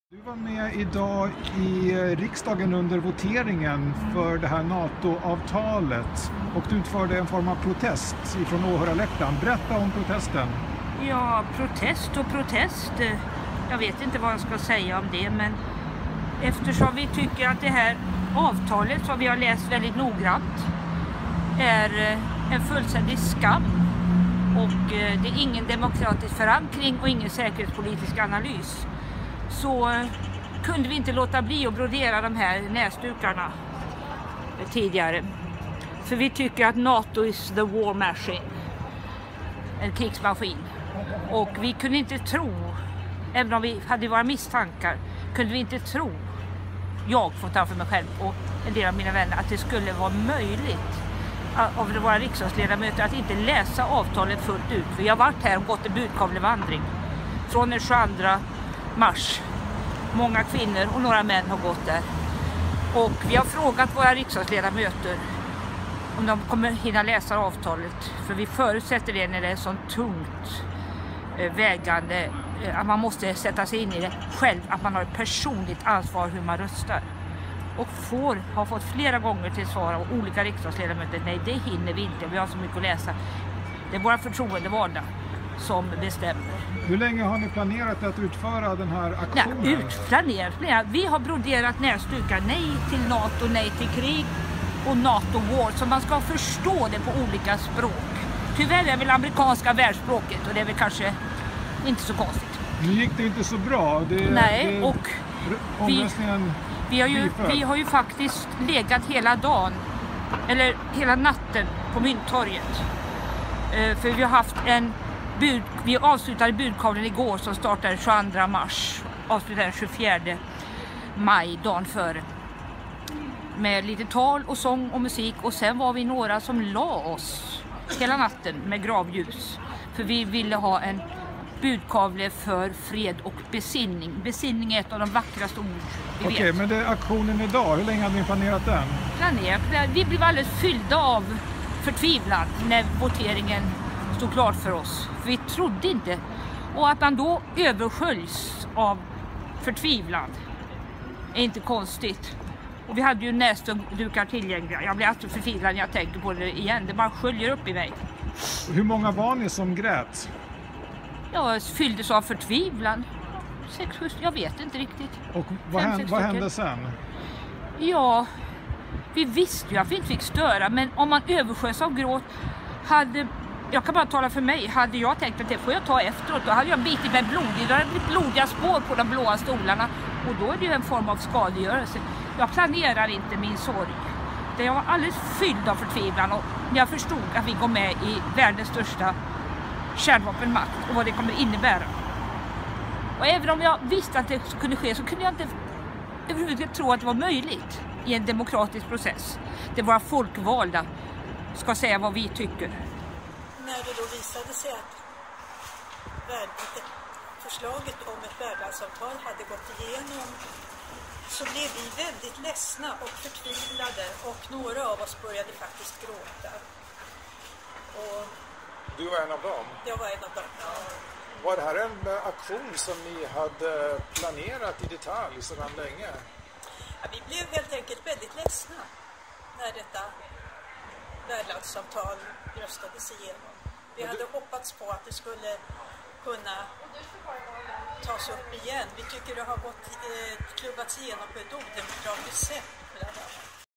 My deltog i gråtprotesten: "Fylld av förtvivlan"
Idag sa riksdagen ja till Natoavtalet. Beslutet möttes av gråtprotester från läktaren, vilket fick talmannen att kalla in vakter.